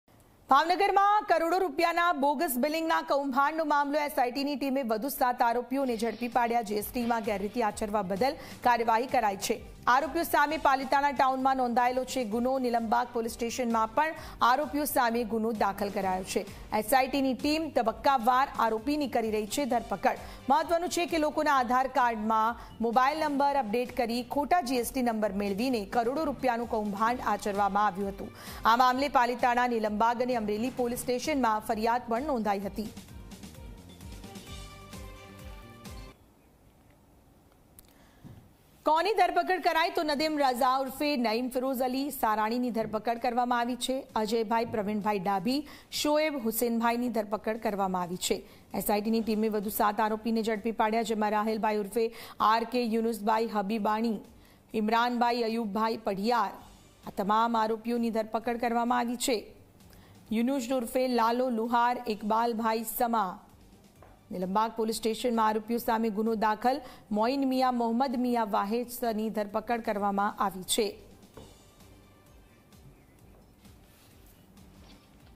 Bhavnagar : કરોડો રૂપિયાના બોગસ બિલિંગના કૌભાંડ | SITની ટીમે વધુ 7 આરોપીઓને ઝડપી પાડ્યા
ભાવનગર: કરોડો રૂપિયાના બોગસ બિલિંગના કૌભાંડનો મામલો...

SITની ટીમે વધુ 7 આરોપીઓને ઝડપી પાડ્યા...

GSTમાં ગેરરીતિ આચરવા બદલ કરાઈ કાર્યવાહી...

આરોપીઓ સામે પાલીતાણા ટાઉનમાં નોંધાયેલો છે ગુનો...

નીલમબાગ પોલીસ સ્ટેશનમાં પણ આરોપીઓ સામે ગુનો દાખલ...

SITની ટીમ તબક્કાવાર આરોપીની કરી રહી છે ધરપકડ...

મહત્વનું છે કે લોકોના આધારકાંડમાં મોબાઈલ નંબર અપડેટ કરીને ખોટા જીએસટી નંબર મેળવીને કરોડો રૂપિયાનું કૌભાંડ આચરવામાં આવ્યું હતું... આ મામલે પાલીતાણા, નીલમબાગ અને અમરેલી પોલીસ સ્ટેશનમાં ફરિયાદ નોંધાઈ હતી...